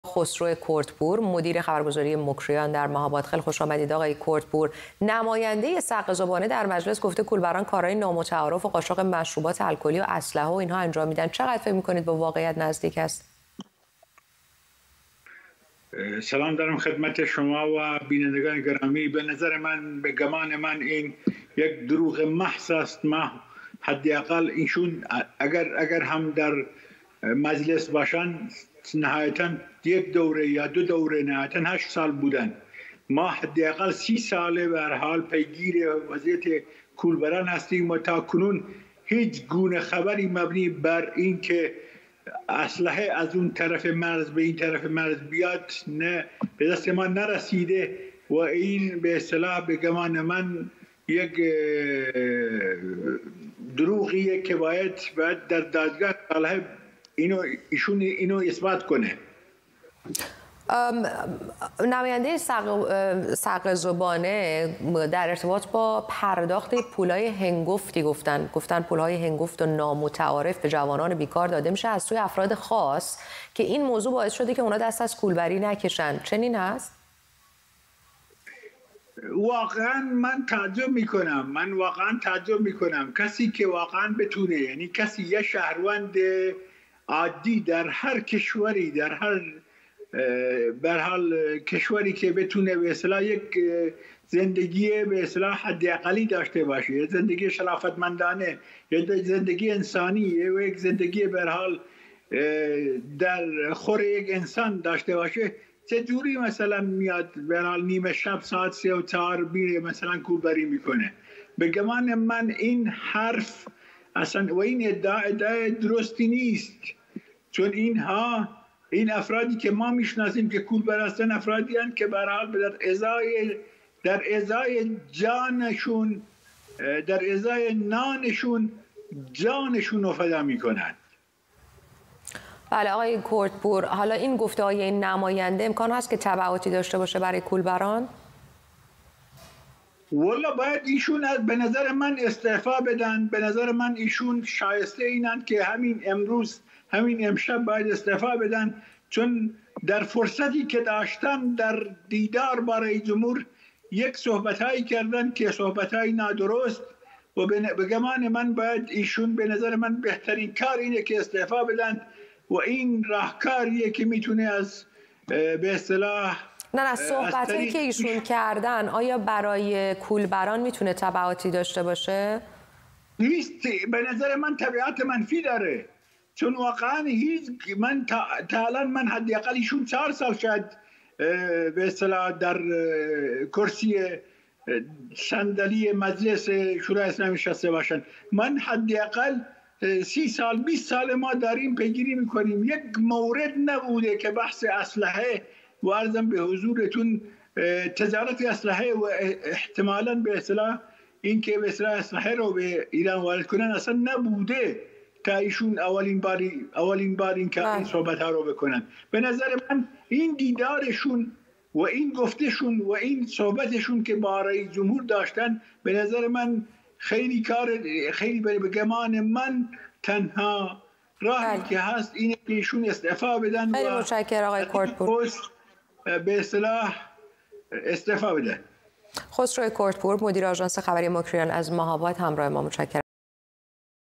واکنش‌ها به ادعای نماینده سقز در مجلس در ارتباط با قاچاق مشروبات الکلی از سوی کولبران
گفت‌وگو با خسرو کردپور، مدیر خبرگزاری موکریان